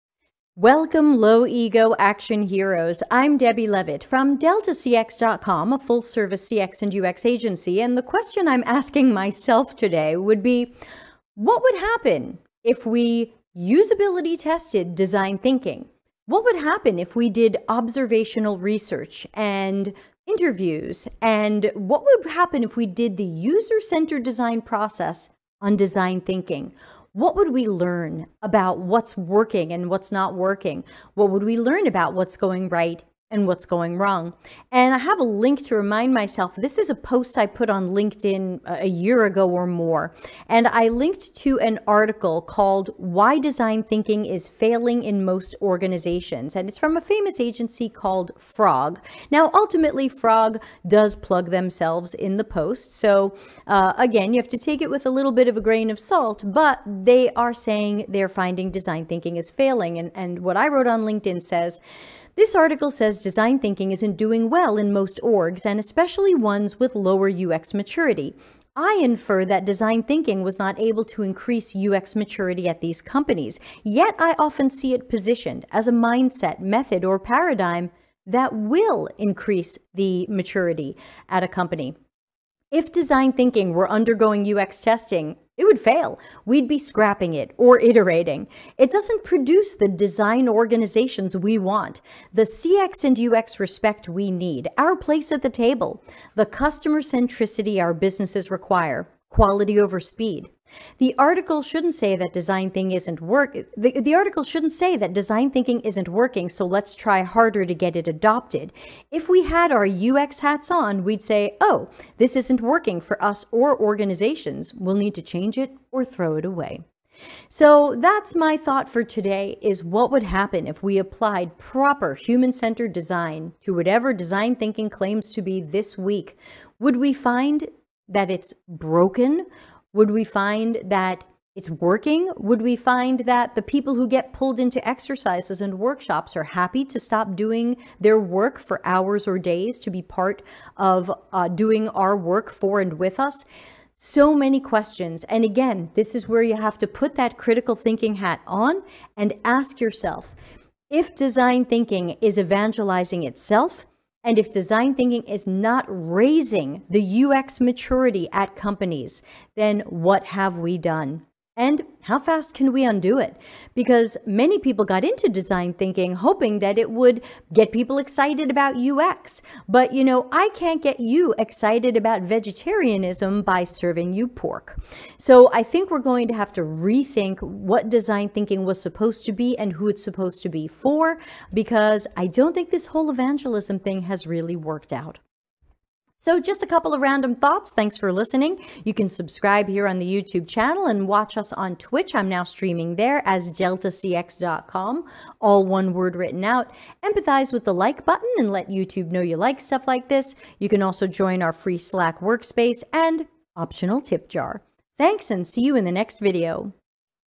Does design thinking increase UX maturity & respect for CX? Or does DT only evangelize itself?
And what would happen if we applied good UCD - research, usability testing, etc. - to design thinking? What would we learn about what's truly going right and wrong with design thinking?